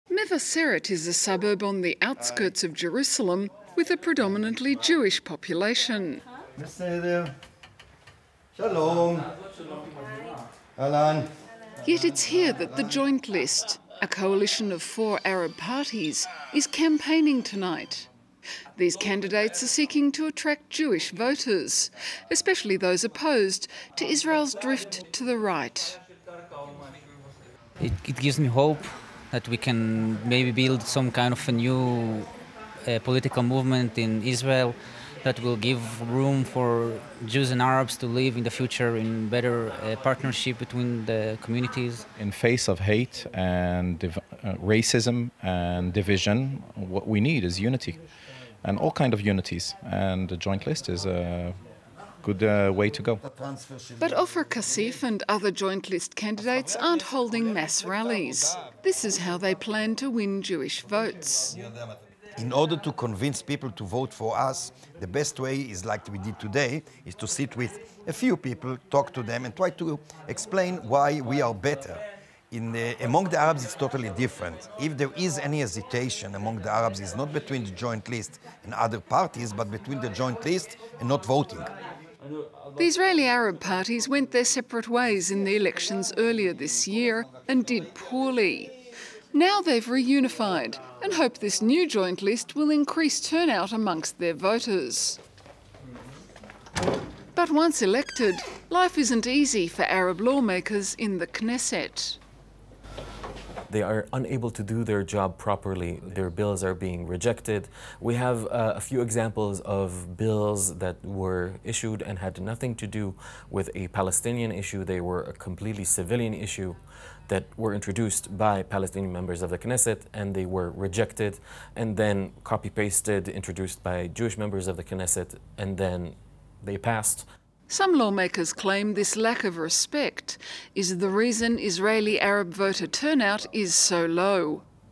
‘What we need is unity’: Israeli Arab coalition appeals to Jewish vote
Scattered and divided only six months ago, Israeli Arab parties are standing united in Tuesday's election re-run, hoping to energise their support base and woo Jewish voters too. FRANCE 24's reporters followed them on the campaign trail.